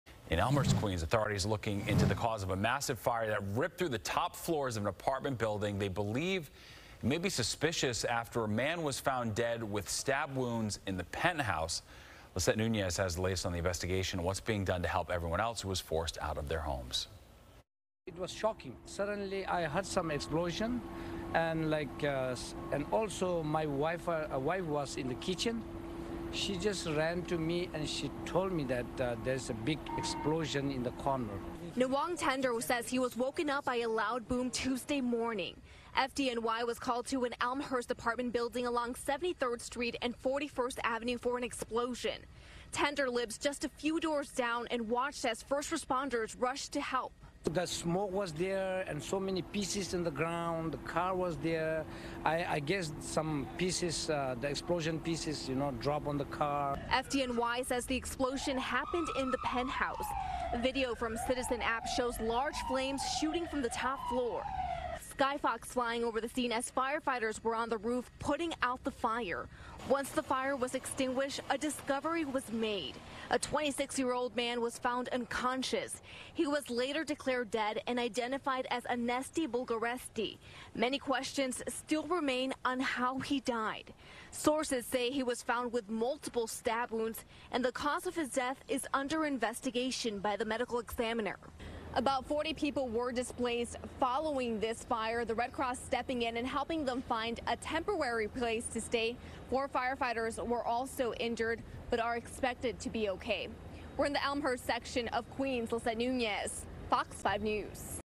Explosion at Queens apartment leaves 1 dead, dozens displaced
Authorities are looking into the cause of a massive explosion in Elmhurst, Queens that ripped through the top floors of an apartment building.